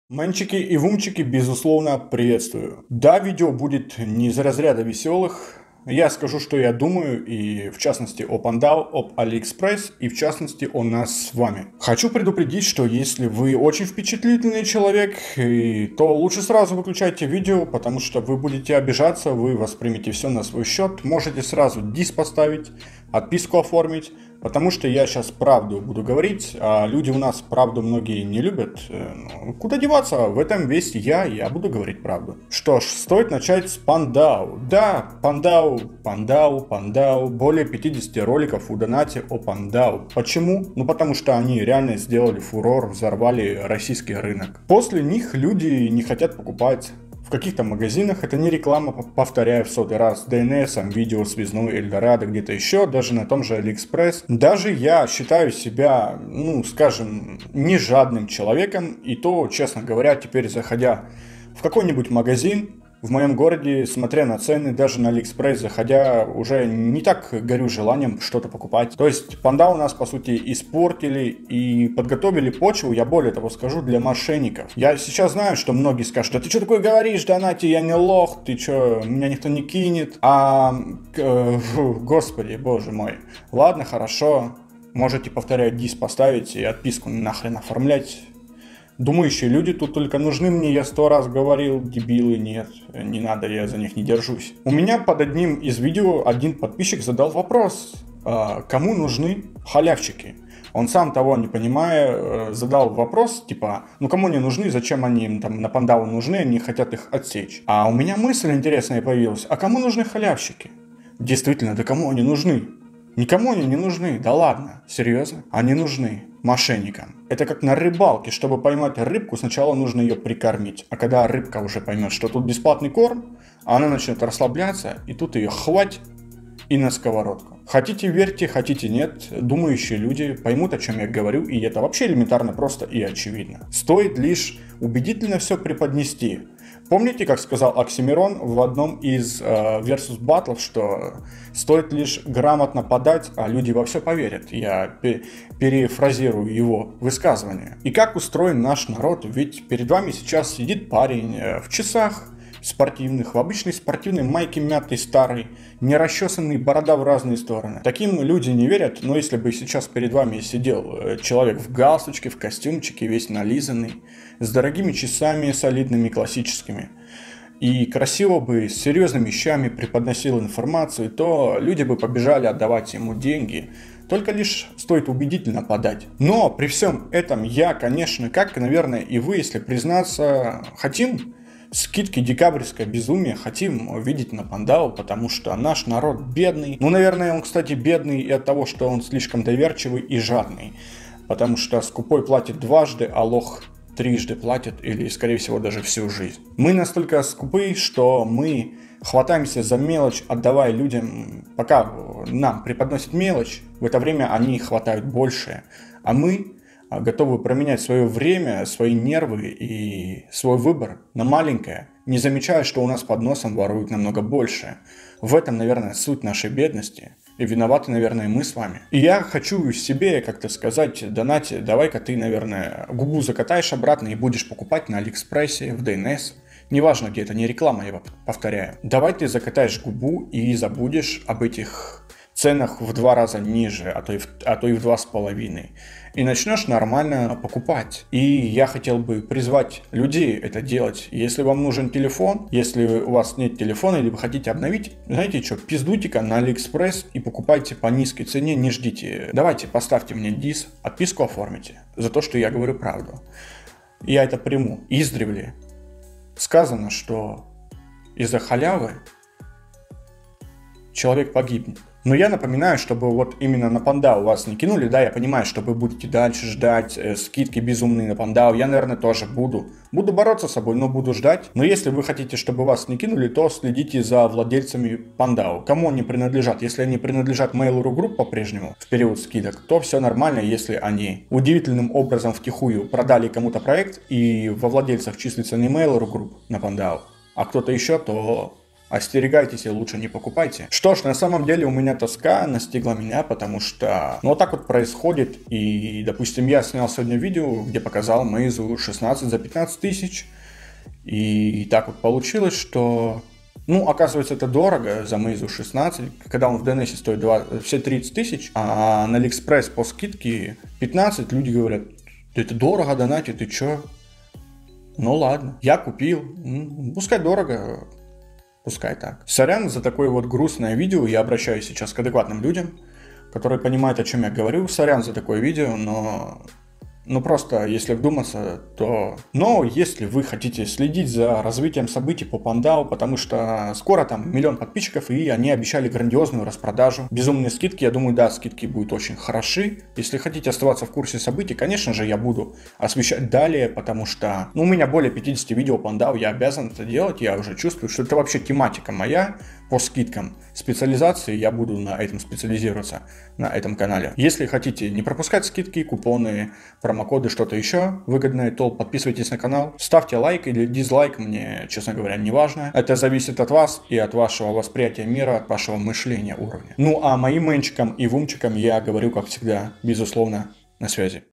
ДАНАТИ: ПАНДАО ОБМАНЫВАЕТ? ПАНДАО РАЗВОД? СКИДКИ НА PANDAO 2019
Все это время Пандао обманывают нас? Пандао это развод? Pandao больше нет для меня? Как так случилось, что скидки на Пандао в 2018 и 2019 году изменили нашу жизнь? Что будет дальше? Поговорим о Пандао, об AliExpress, о скидках на Pandao и о скидках на АлиЭкспресс "Нам 9 лет".

Также затронем тему о мошенниках в интернете, как мы сами готовим себя к пирамиде, хайпу и т.д. Как мы сами клюем на приманку и позволяем себя подкармливать, приучая себя к "волшебству", "чуду", и "магии" :) Приучаем себя быть через-чур доверчивыми, наивными и тем самым, ставим себя в опасное положение в будущем.